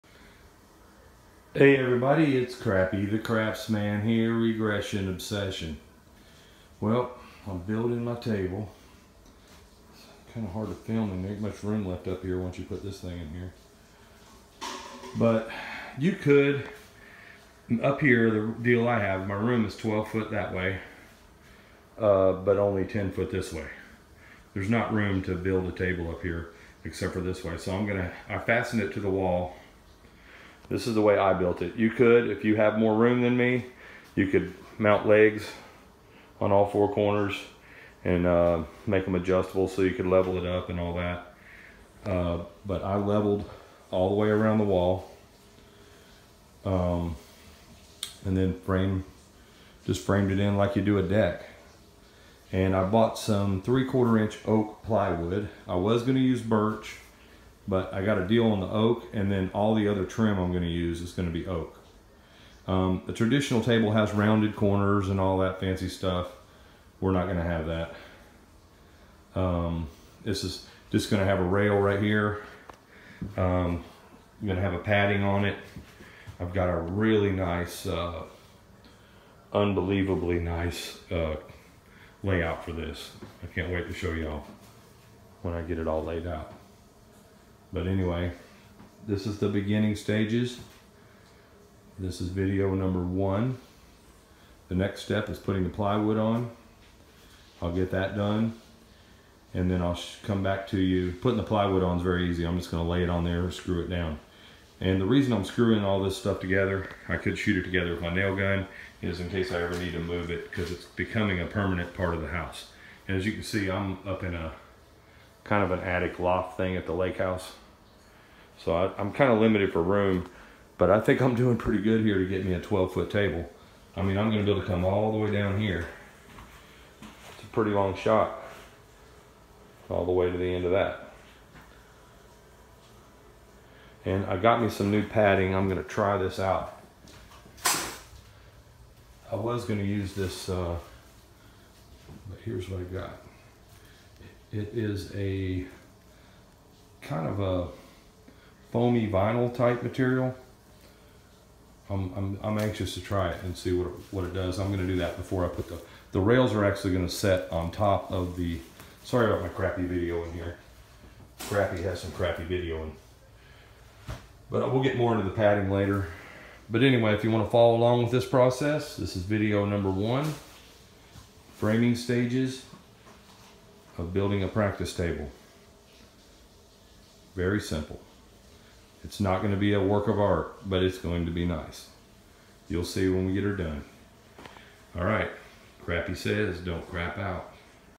How to build a simple craps practice table part 1 framing
Part 1 of 7  build your own craps practice table step by step